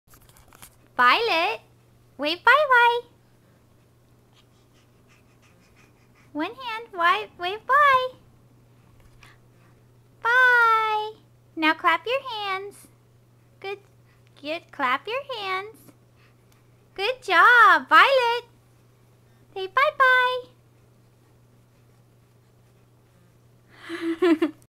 Violet waves hi and bye bye
Violet learned to wave hi and bye-bye today!